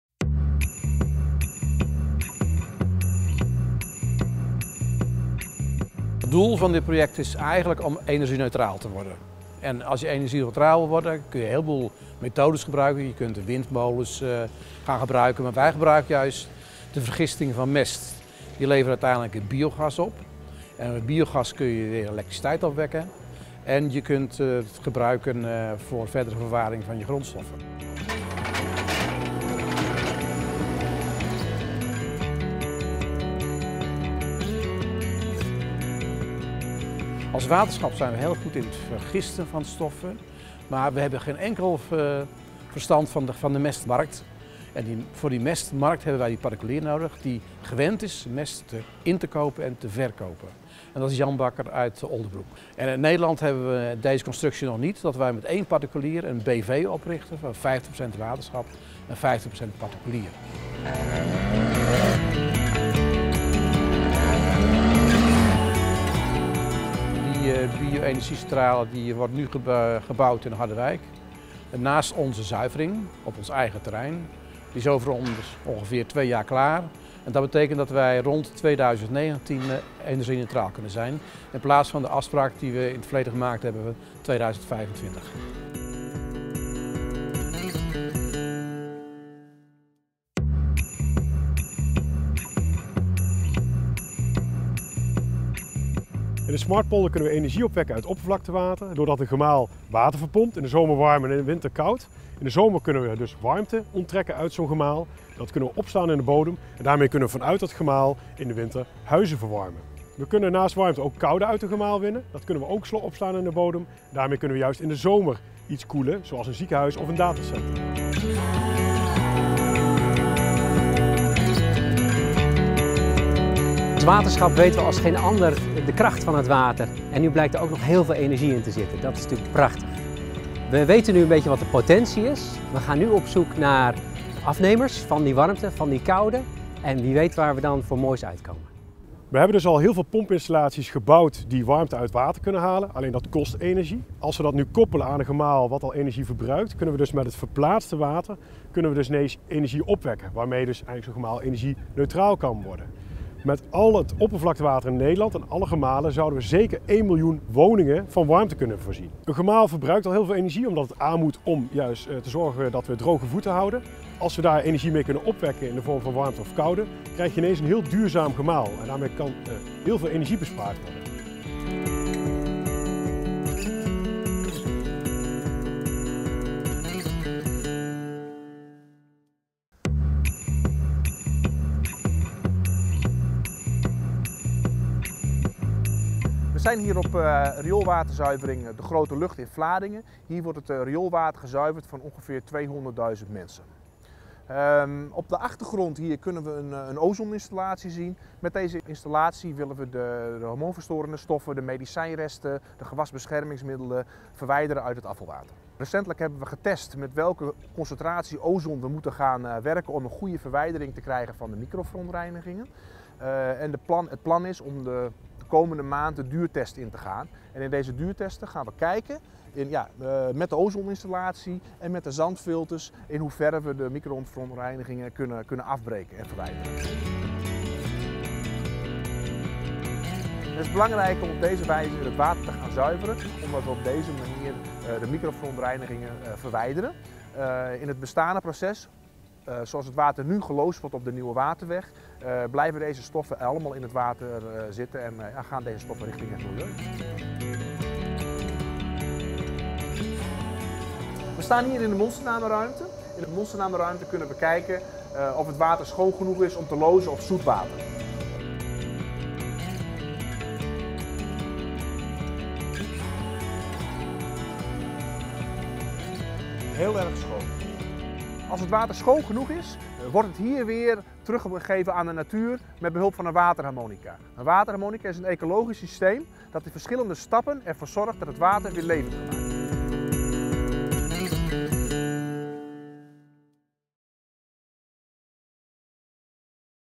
Schoon water: Genomineerden Waterinnovatieprijs 2016
Waterschappen zuiveren het afvalwater van huishoudens en bedrijven. Afvalwater bevat waardevolle grondstoffen en energie. Dit zet de deur open voor veel innovaties. Zie hier de genomineerden voor de Waterinnovatieprijs 2016 in de categorie 'Schoon water'.